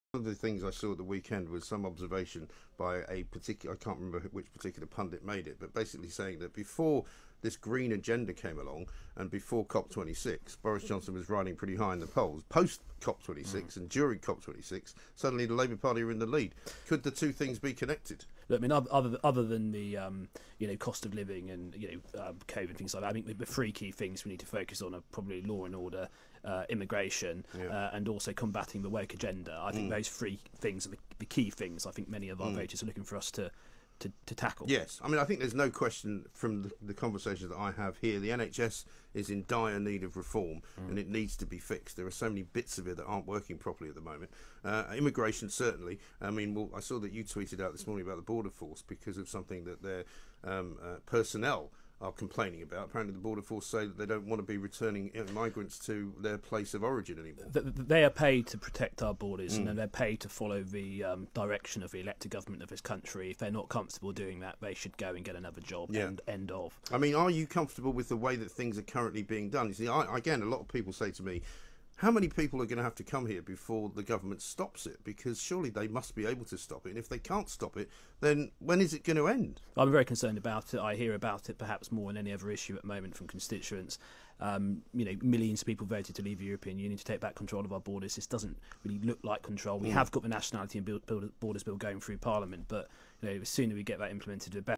Mike Graham
On Talk Radio this morning I told Mike Graham that the three key things we need to focus on are law and order, immigration and combatting the woke agenda.